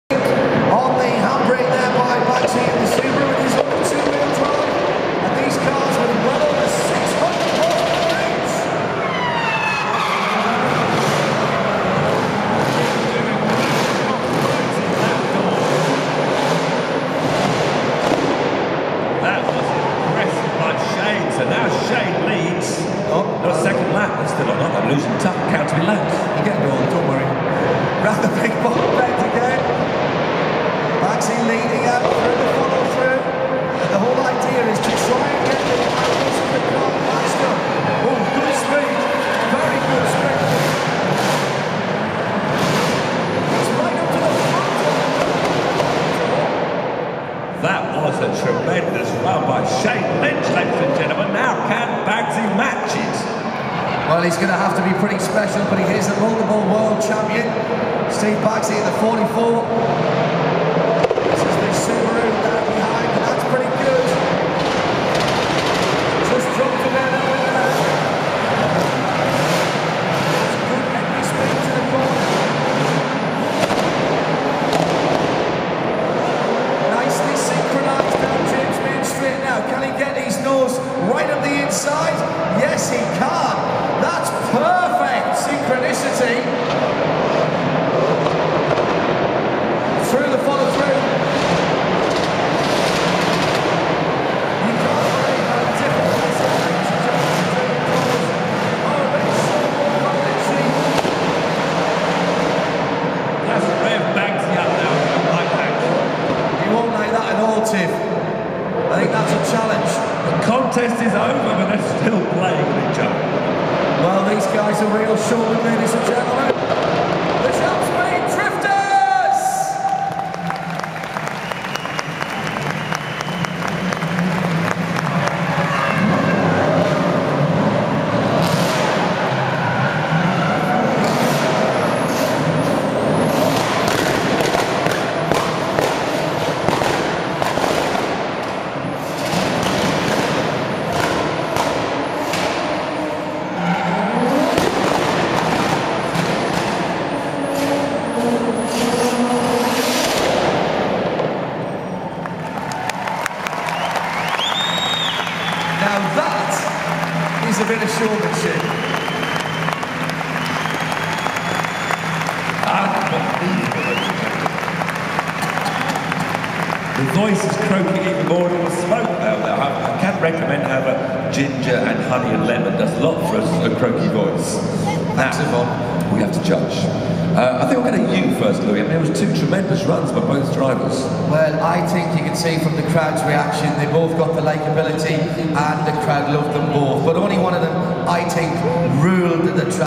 Shane Lynch And Steve Baggsy Biagioni Drifting At Top Gear Live
Drifting At Top Gear Live (London 2011)